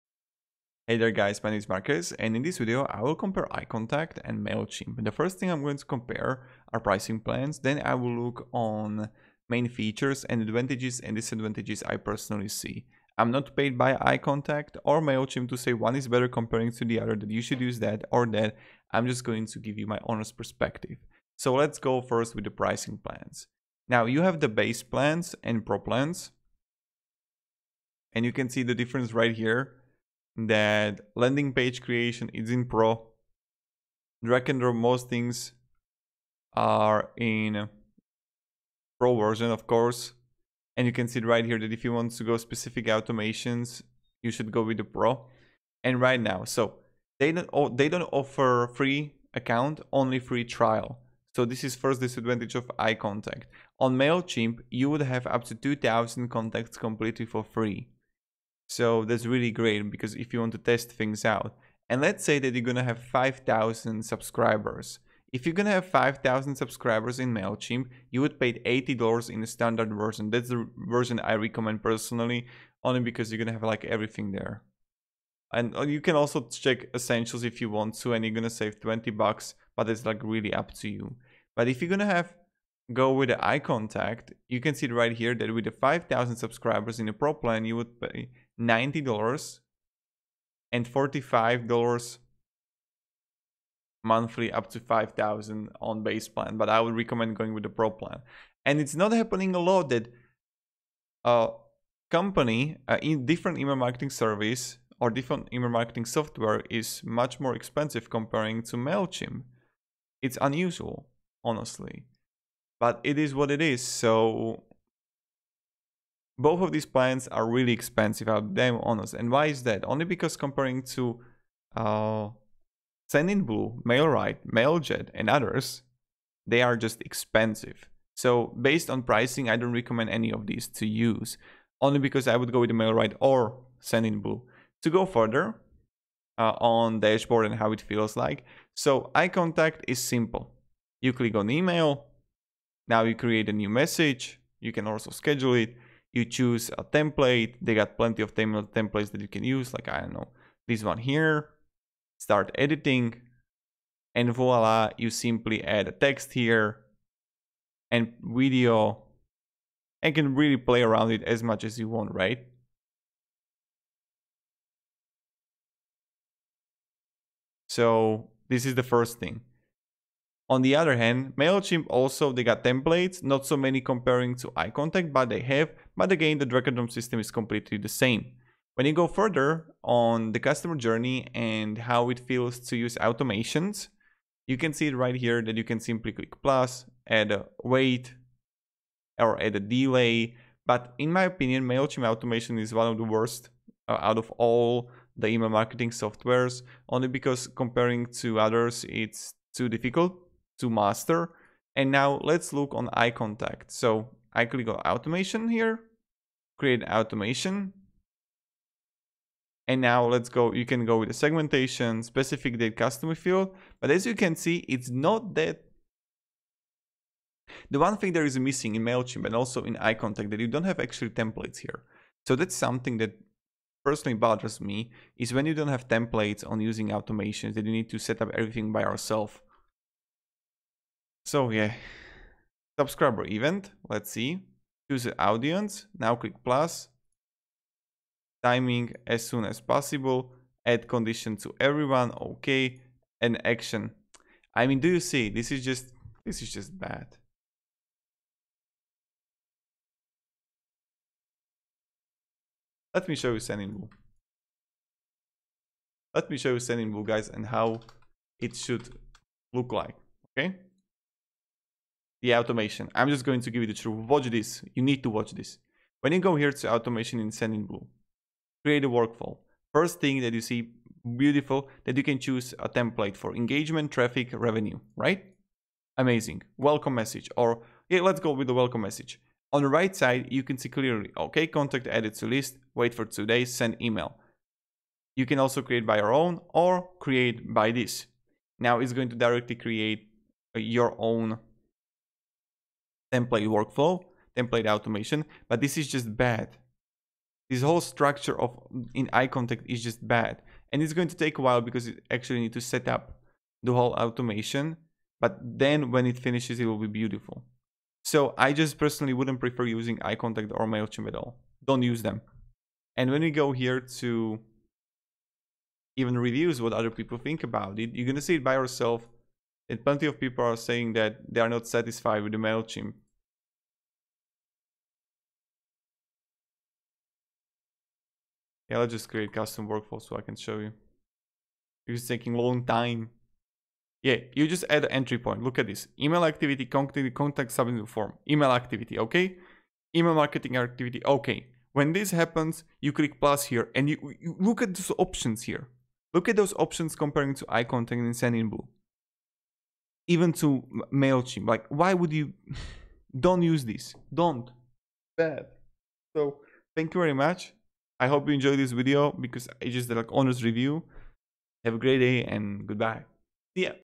Mailchimp vs iContact - Which is Better?
In this video I will show you comparison between Mailchimp vs iContact

GET AMAZING FREE Tools For Your Youtube Channel To Get More Views:
Tubebuddy (For GROWTH on Youtube):